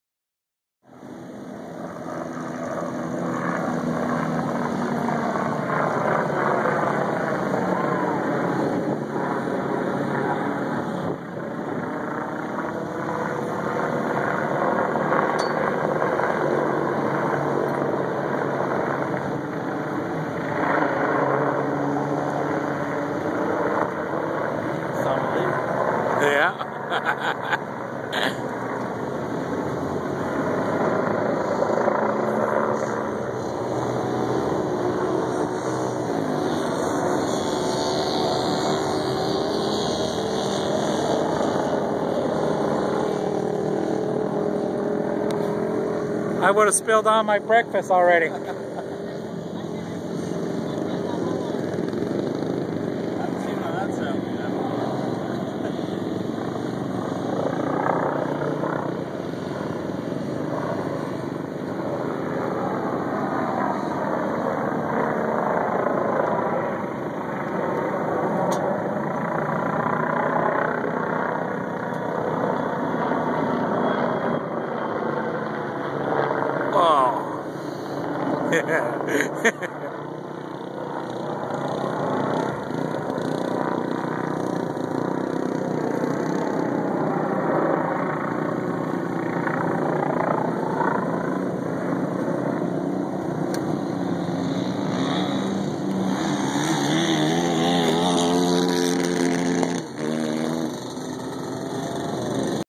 RED BULL HELICOPTER FLIPS AT GLAMIS 2013
red bull helicopter doing stunts at glamis over thanksgiving weekend 2013